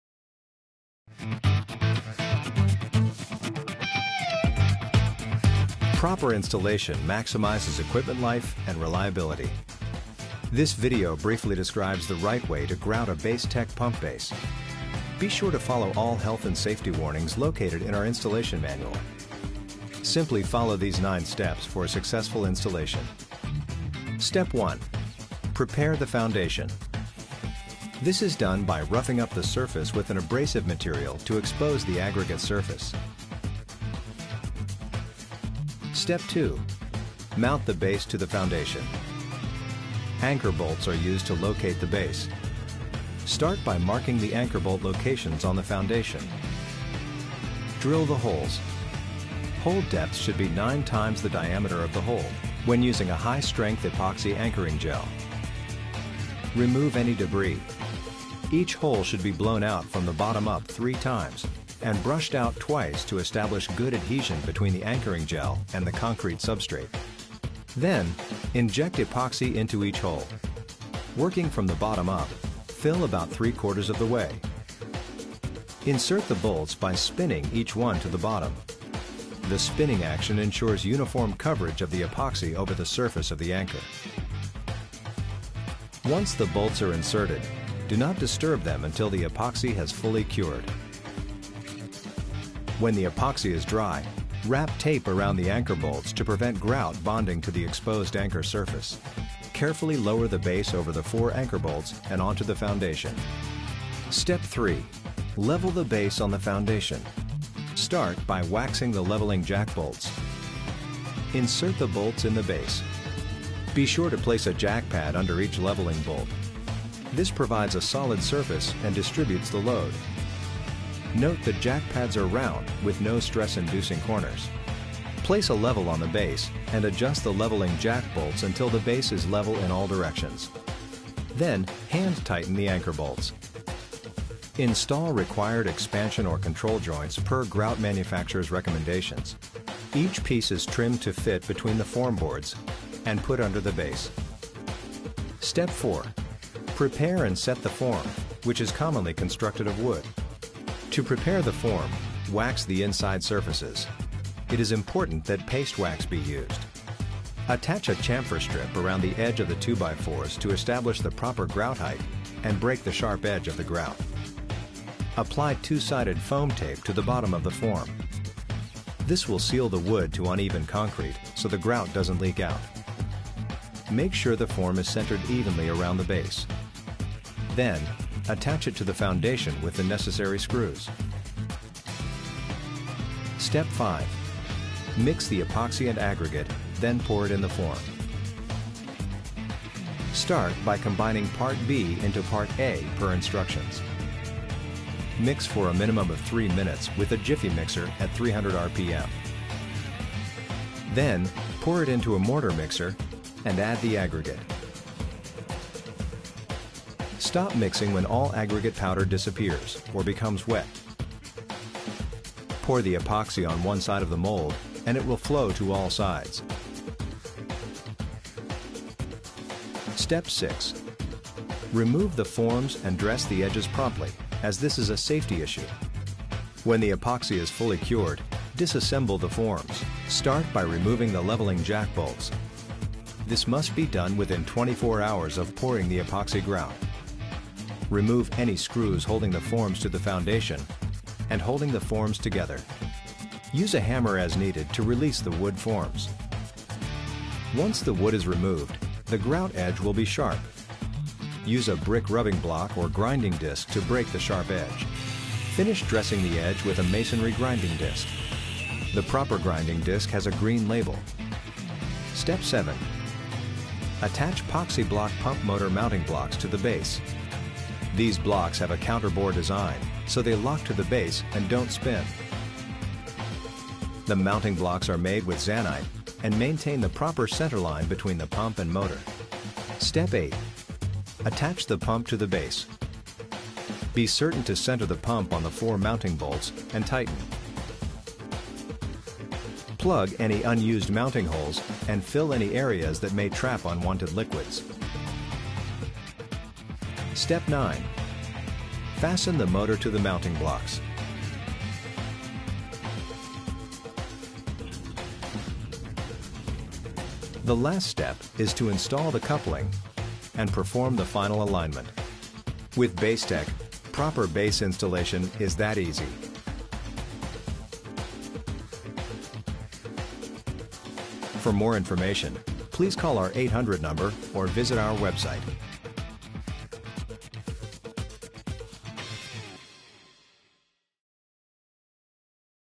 PoxyBase Installation
How to install a BaseTek PoxyBase pump foundation (grouted option)